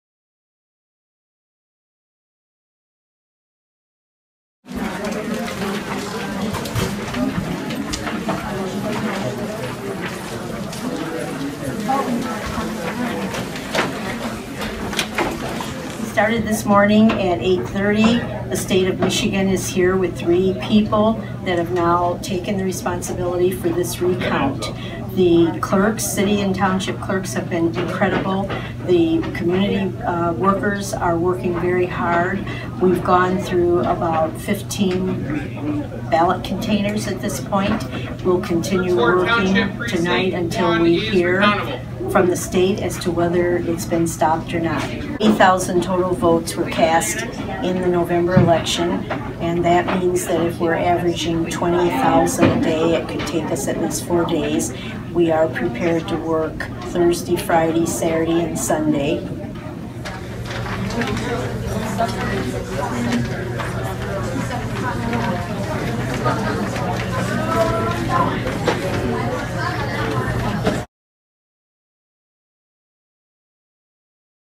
Muskegon County presidential recount 12.7.2016
Muskegon County Clerk, Nancy Waters, talks about recounting the 80,000 Muskegon County ballots on Wednesday, Dec. 7, 2016 in Laketon Twp., Mich.